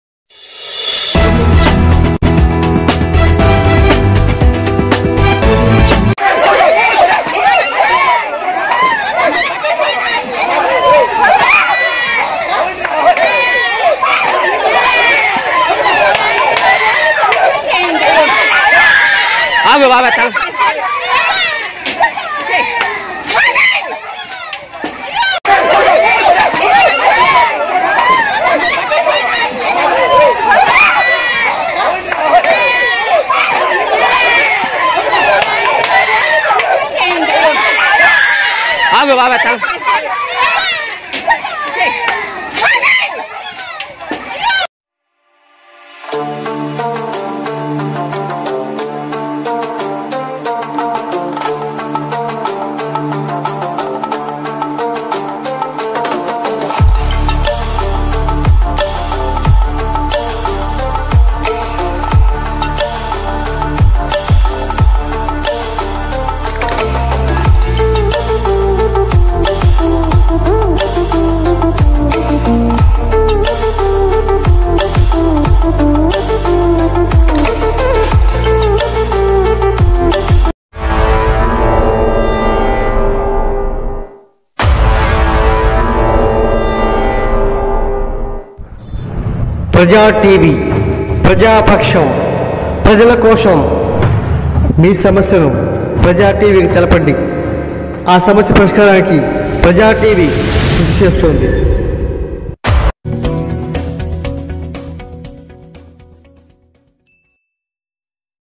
గుంటూరు జిల్లాలో టిడిపి,వైసిపి వర్గాలు కర్రలతో దాడులు. Praja tv
నాదెండ్ల మండలం,చిరుమామిళ్ళ గ్రామంలో జరిగిన సంఘటన. Praja tv